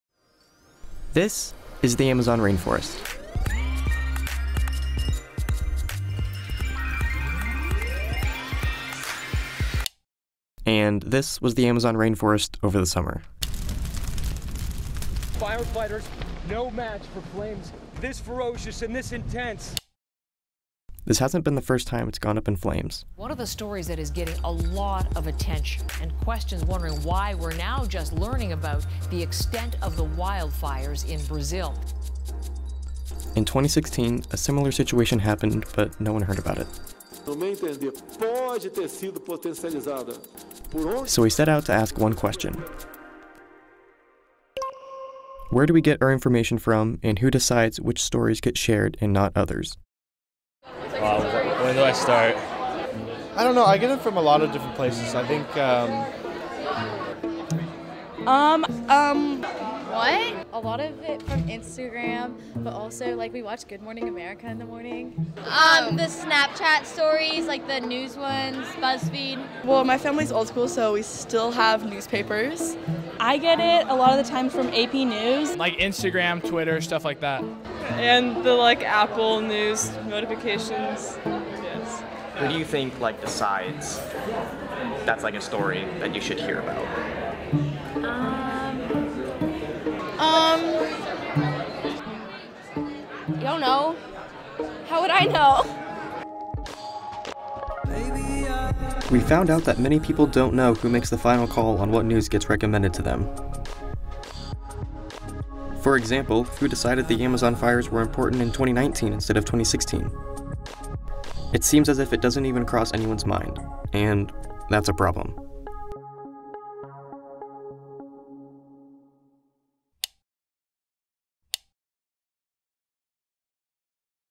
Social Media's Effect on the Amazon Rainforest Fires
Over the last couple years the amazon rainforest has been on fire. In 2016, the fires were just as bad as 2019. The real question is why were the amazon rainforest fires in 2019 a bigger deal than the fires in 2016? It seems like one factor may be the growing use of social media. Social media allows people all over the world to connect and communicate on whatever they want. In this video, you'll learn about the younger generation's use of social media, and how they get their news.